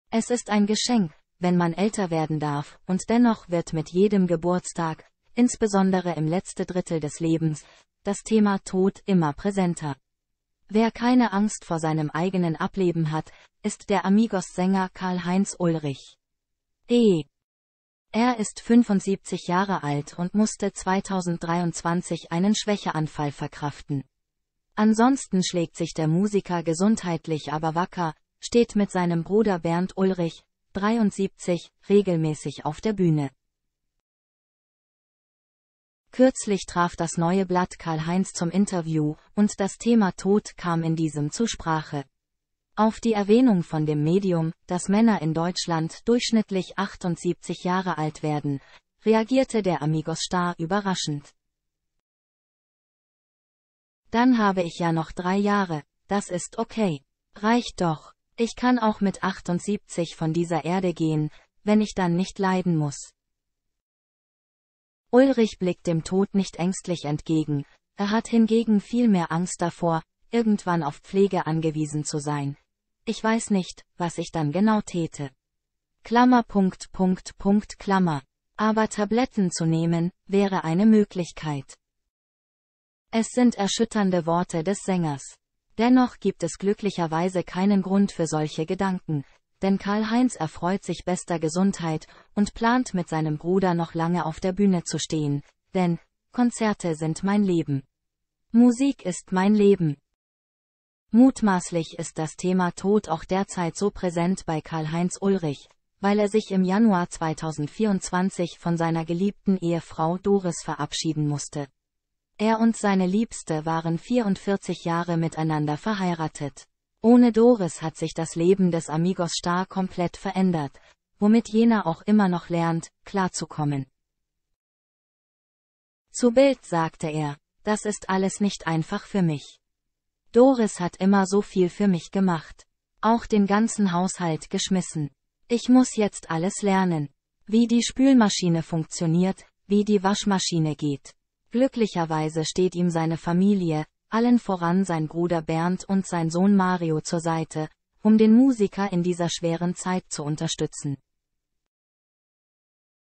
Amigos-Star Karl-Heinz Ulrich: Traurige Worte übers Sterben - "Tabletten wären eine Möglichkeit"
Es sind traurige Worte von Die-Amigos-Star Karl-Heinz Ulrich: Wenige Monate nach dem Tod seiner Frau spricht er über das Sterben.